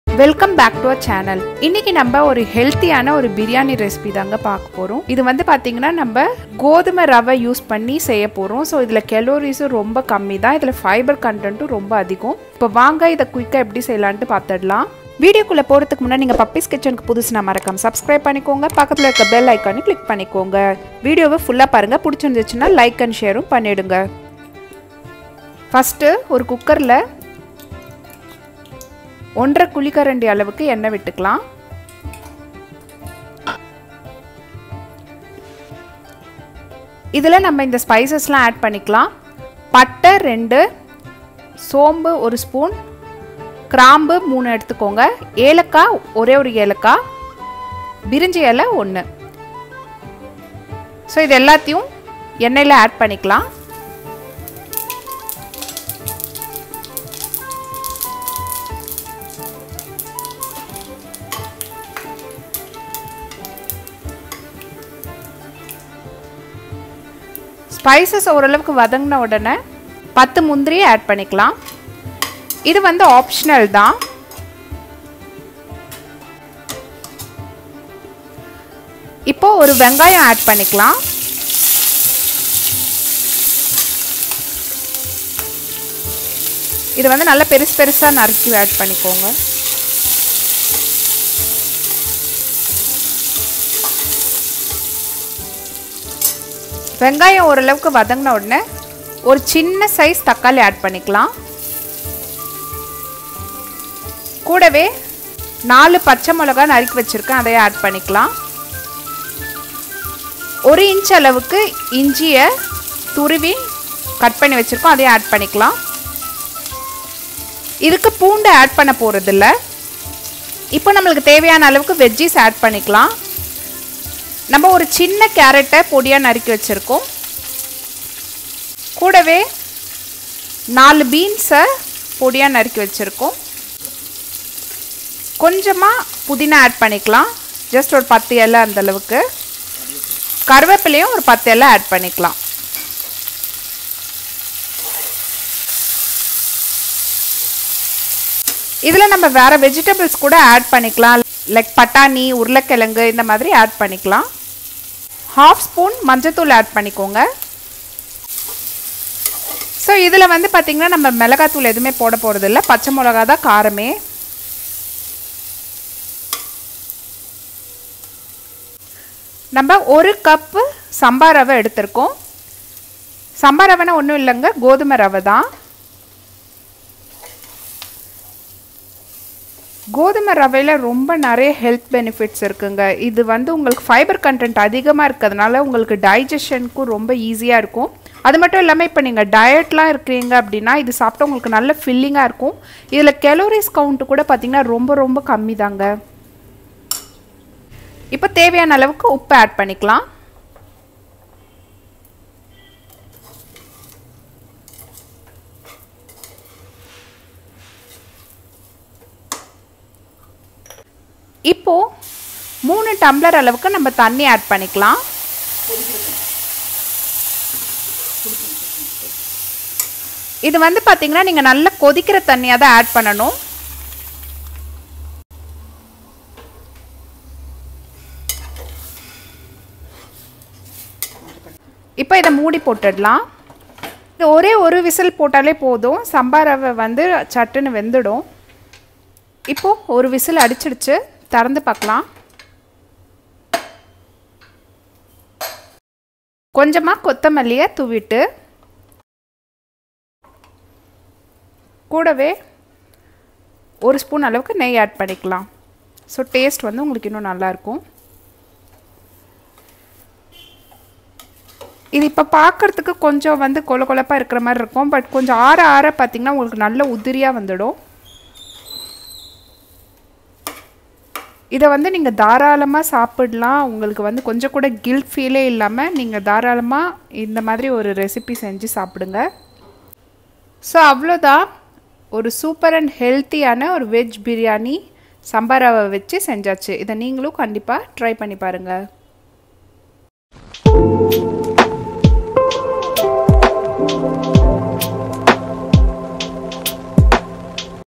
Samba Rava Biryani | Low calorie diet recipe | Puppy’s Kitchen
In this recipe learn to make delicious veg biryani using samba rava also called as wheat rava which is high in fibre so make you full and good for digestion and not just that its having very less calories so we can have this biryani without guilt.

If you are interested in such healthy version of biryani…checkout our Thinai (Fox Millet) biryani recipe